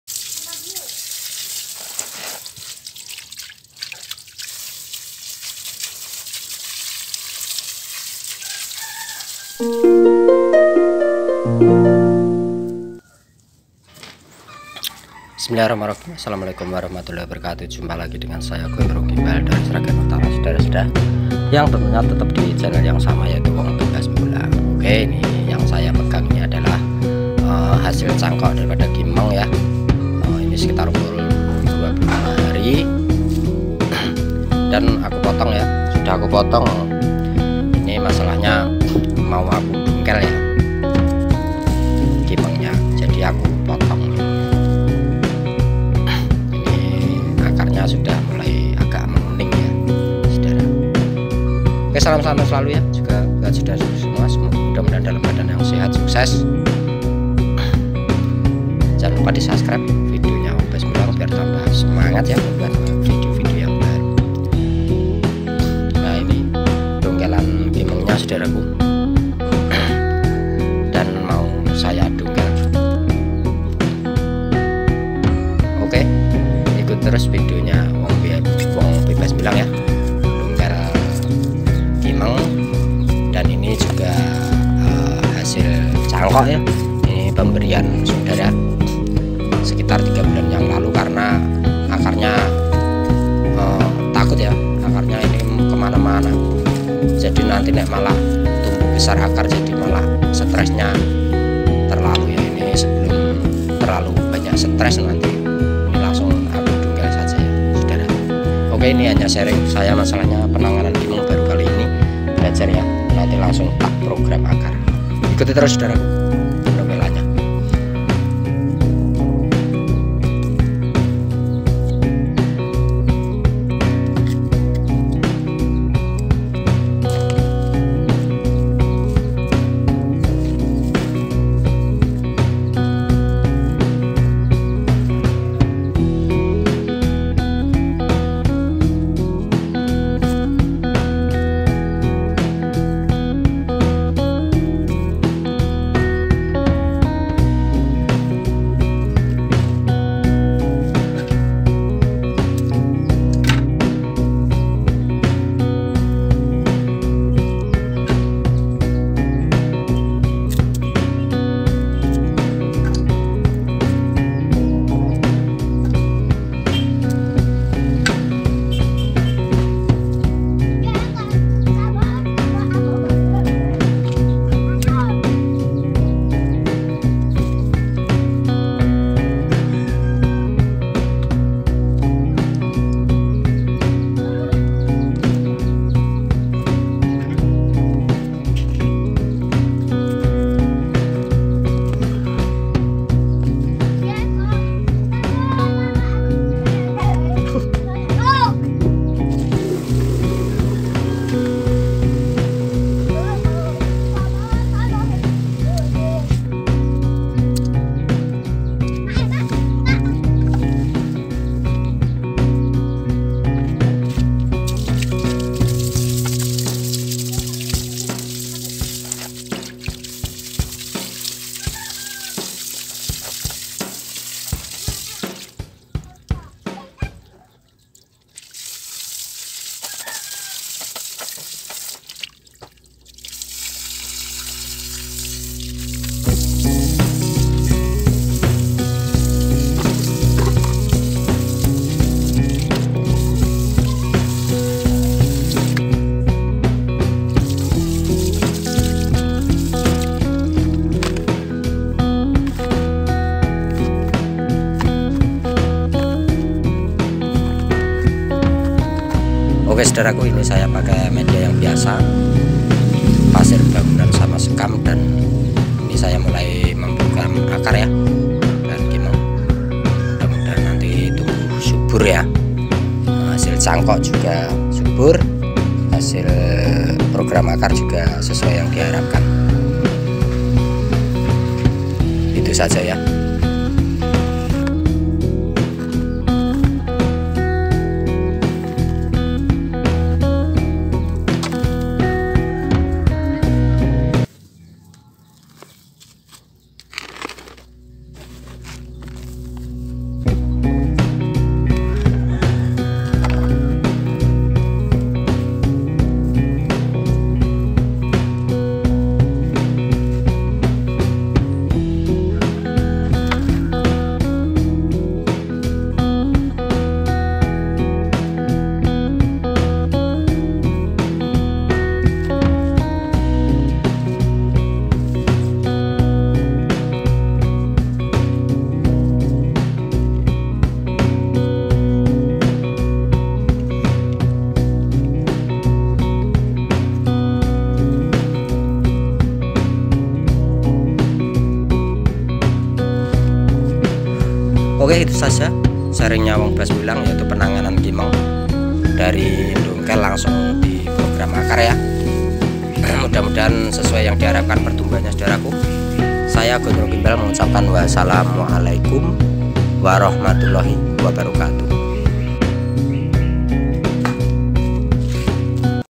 NDONGKEL KIMENG LANGSUNG PROKAR#bahanbonsai#belajarbonsai#wongbebasmbolang
kali ini saya mendongkel kimeng dan langsung aku program akar,kali ini saya belajar menangani bahan bonsai kimeng hanya sharing beebagi pengalaman mudah2n ada manfaat klu ada kekeliruan mohon saran dan kritikan,wongbebasmbolang selalu terbuka karena belajar butuh saran dan kritikan🙏🏻